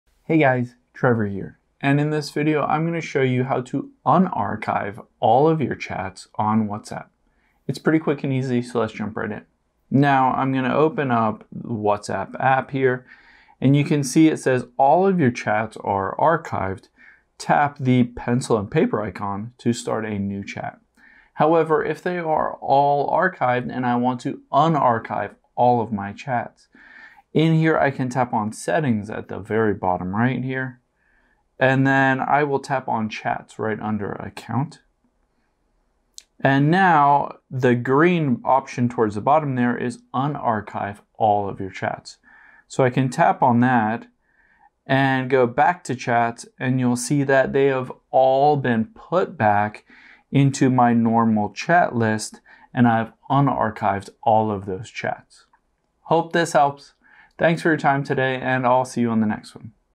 How To Unarchive All Chats On WhatsApp
Let's unarchive any chat messages you previously archived on WhatsApp if you want to talk with the person again.